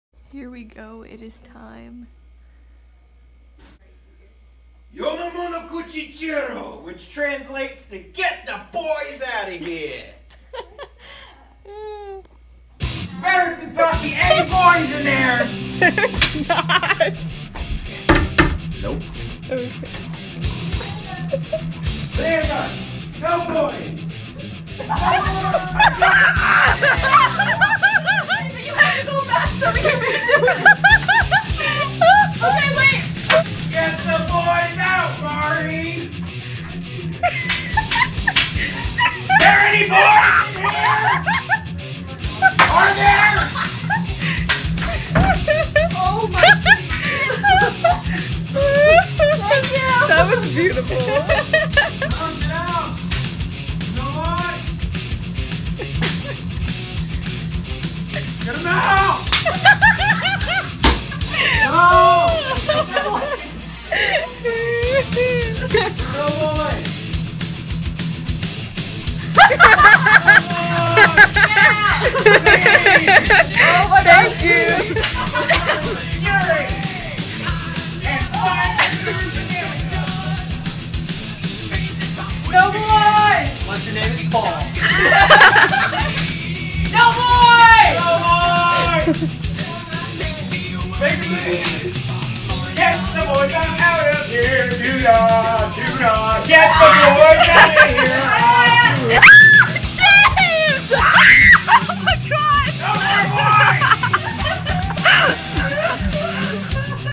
Hilarious "No Boys" Moments! (PVG Girls Dorm, Corban University.)
On weekends, boys are allowed in the girls’ hall after 2pm, but at 10pm, they better get out, or else the PVG RAs will NOT be happy! Lol
{{made for purely entertainment purposes.}} —Residents Of PVG, girls’ hall. ;)